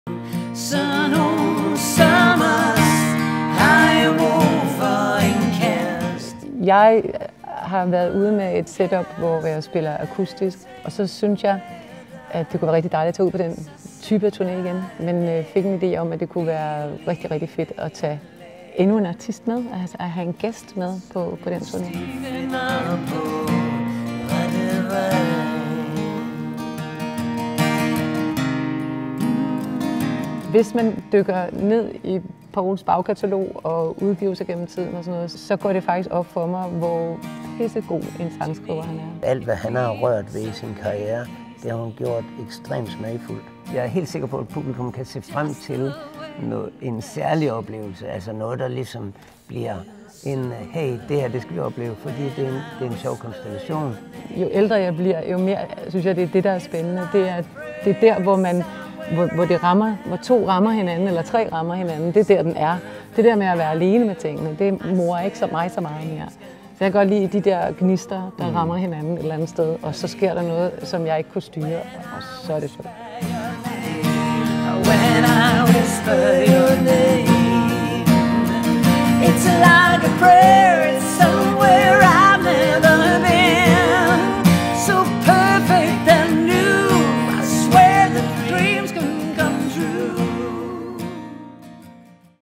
Hanne Boel + special guest Poul Krebs i Vejle Musikteater
Hanne Boel + special guest Poul Krebs i Vejle Musikteater fredag den 5. oktober 2019.